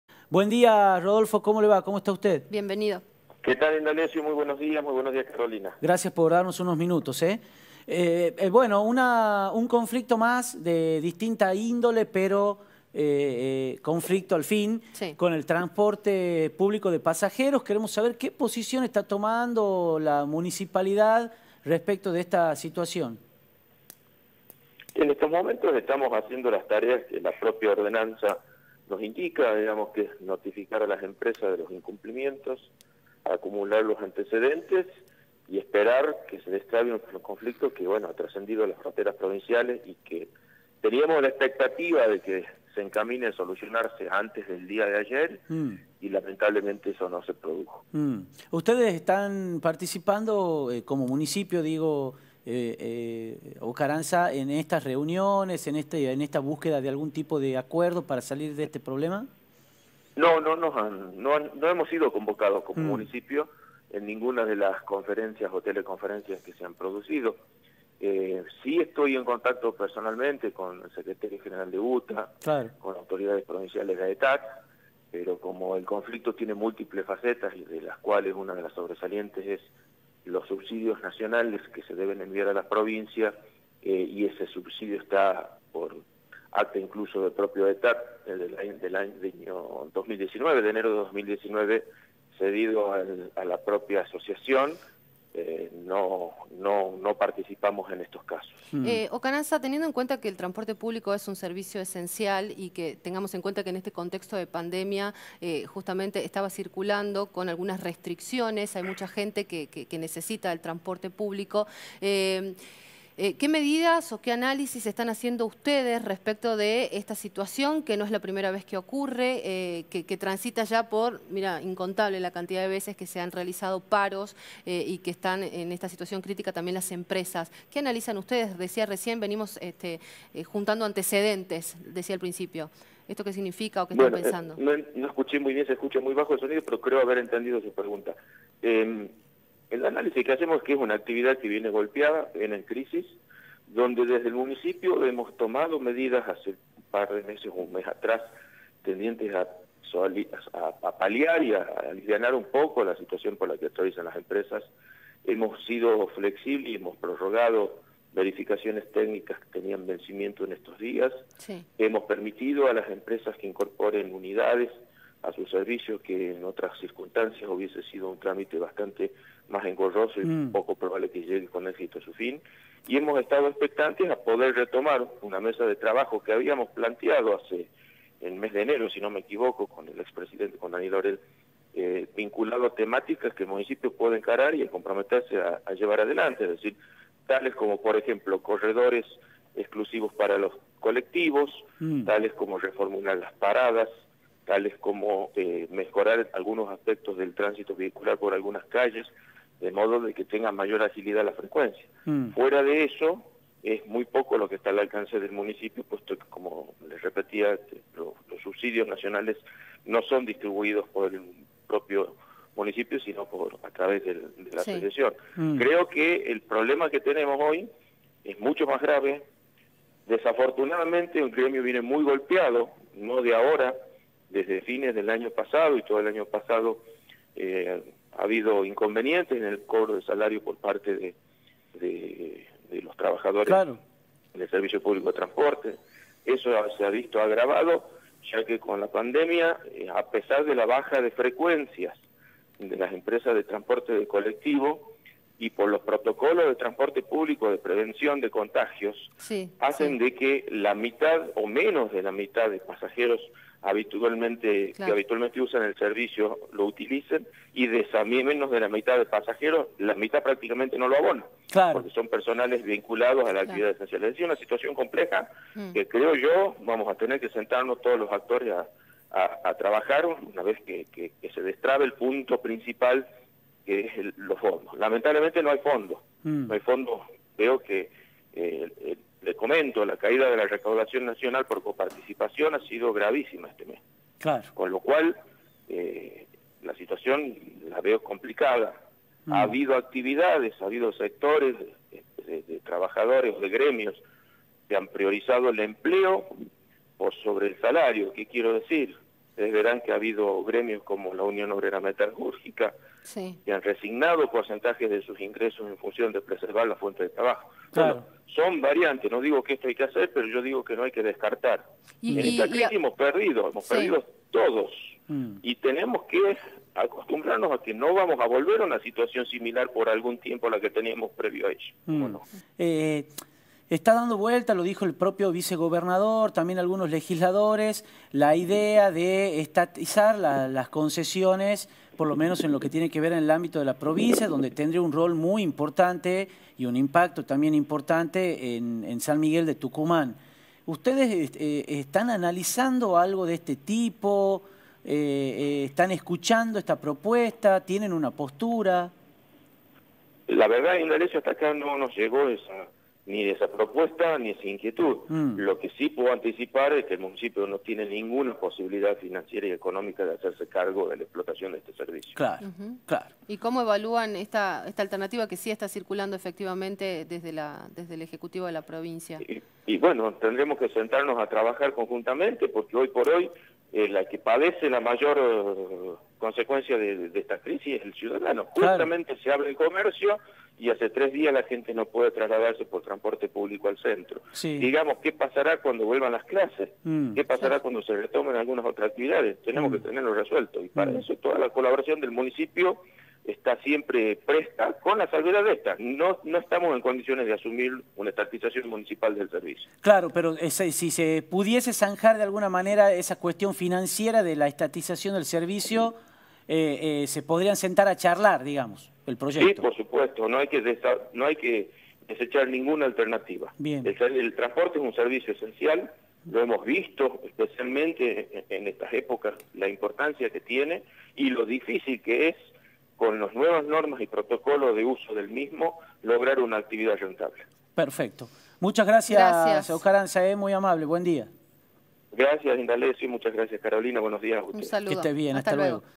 Conflicto del transporte: desde la Municipalidad aseguran que no hay fondos
Entrevista a Rodolfo Ocaranza, Secretario de Gobierno de la Municipalidad de San Miguel de Tucumán.